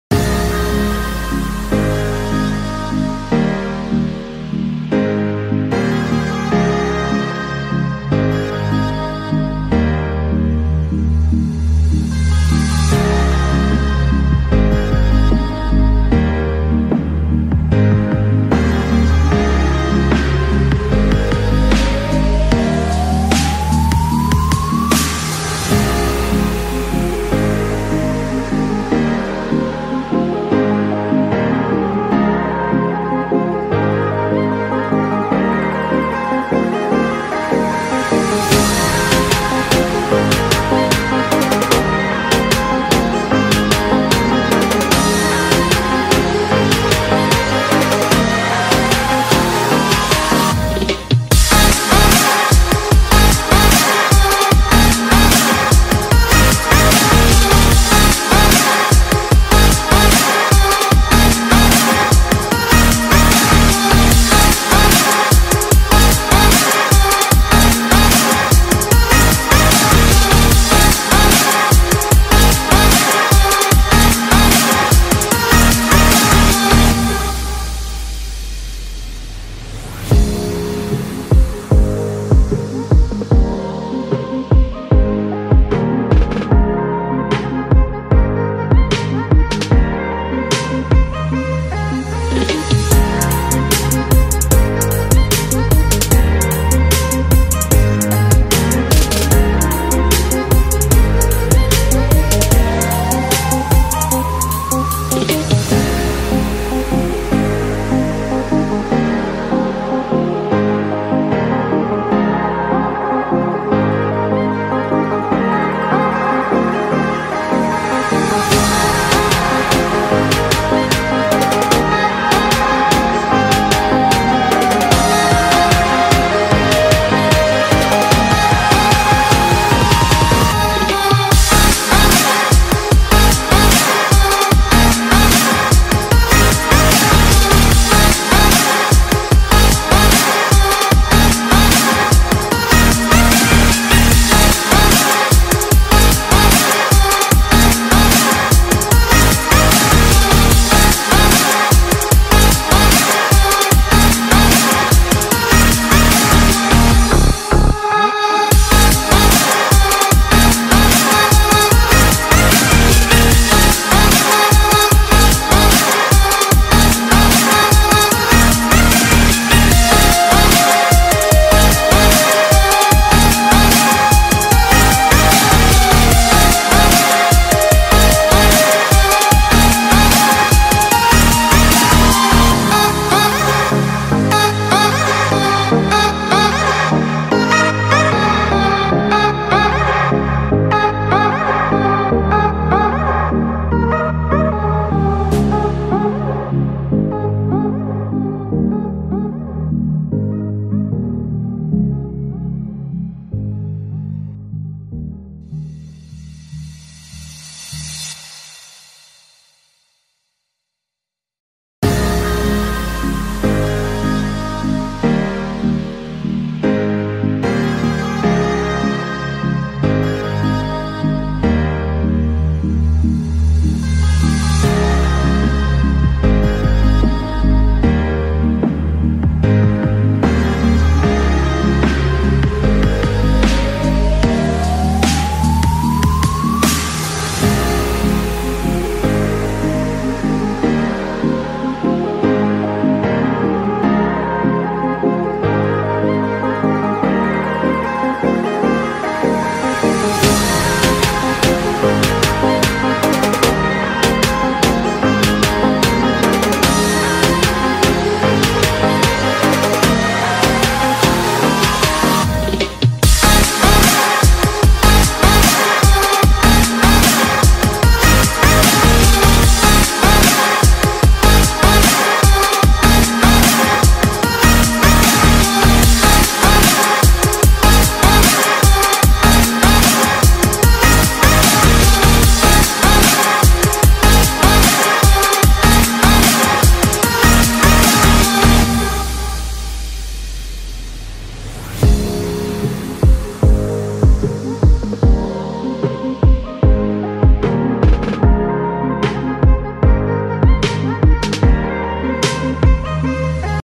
Drawing Arisu ( Kento Yamazaki) | Alice in Borderland [hatching technique]
Hi guys this is my new drawing Kento Yamazi (Arisu) in Alice in Borderland. I used 6b and 8b staedler (black) pencils here. Who do you want me to draw next? Comment it down below 😊❤️